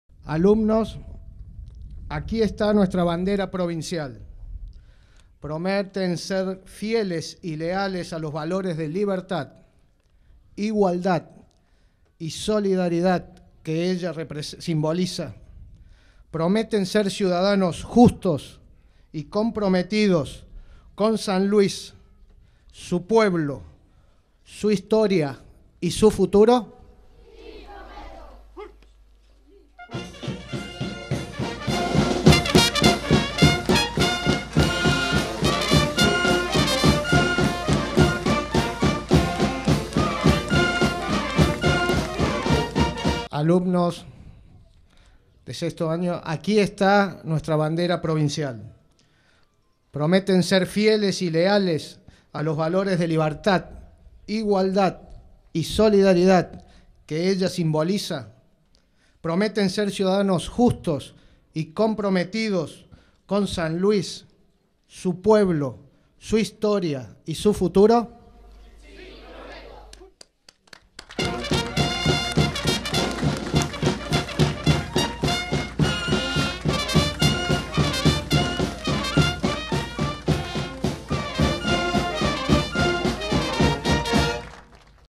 LEALTAD A LA BANDERA DE SAN LUIS
En Buena Esperanza, alumnos de 4to. Grado y de 6 años prometieron lealtad a la bandera de San Luis. Un gran desfile culminó la jornada.